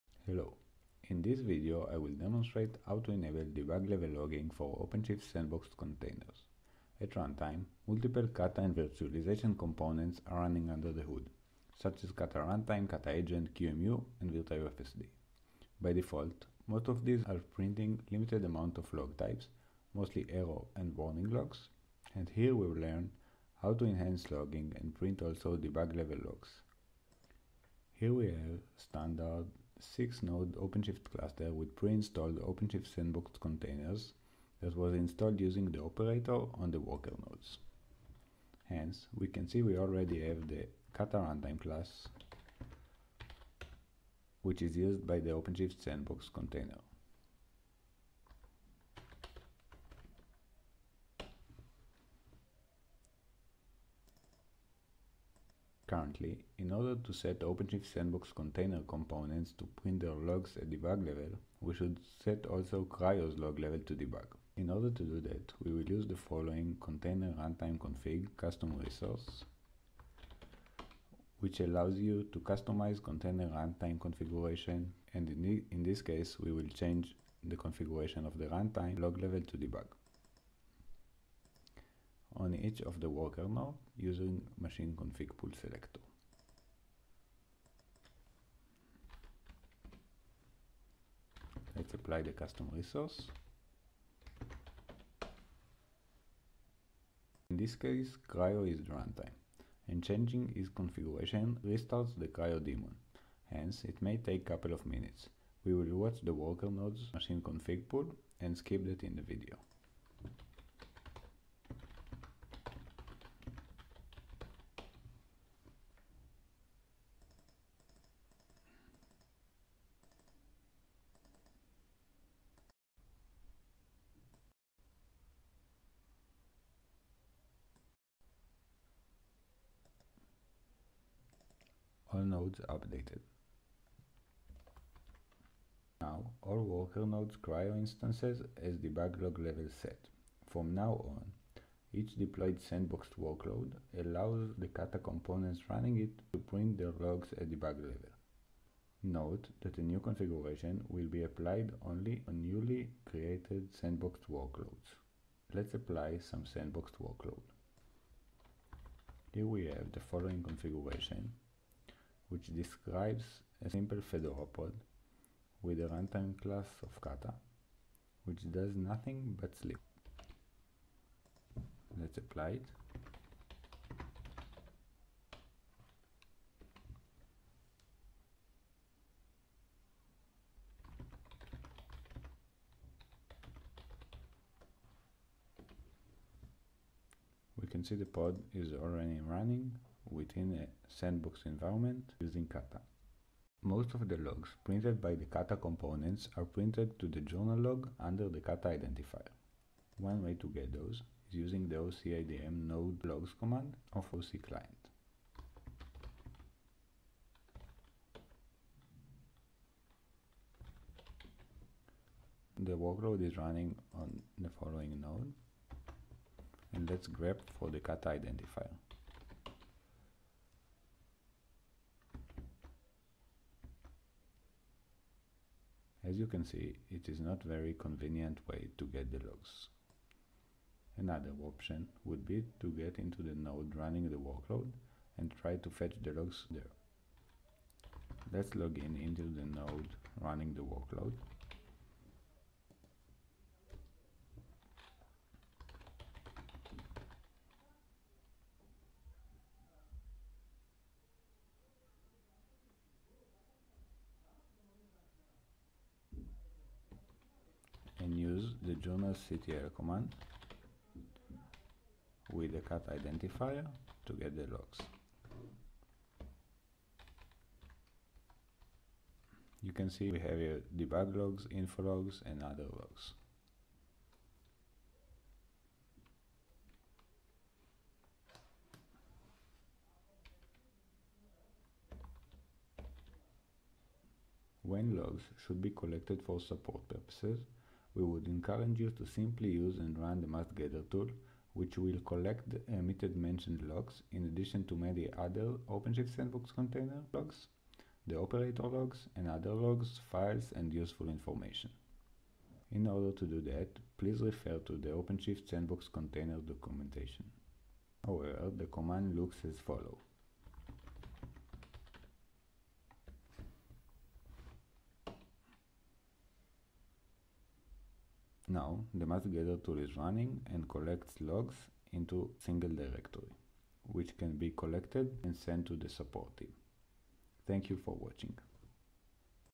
Enable debug log level for Red Hat OpenShift Sandboxed Containers
In this video we'll present how to increase logs level of the different Red Hat OpenShift Sandboxed Containers to "debug", then, the enhanced logging can be viewed in the node's journal or to be collected by the must-gather tool.